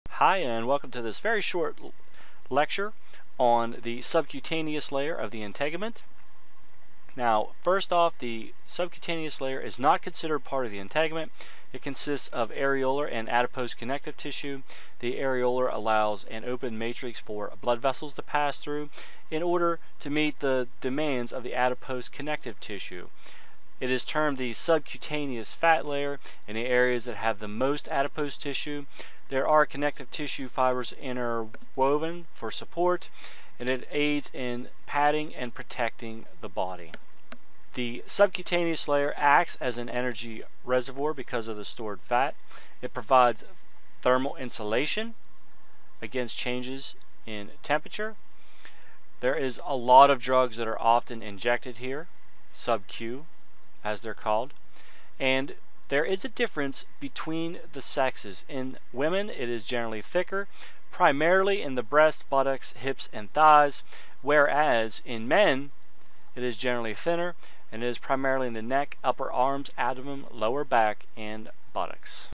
hypodermis anatomy and physiology
A very short whitespace lecture capture describing the anatomy and physiology of the hypodermis.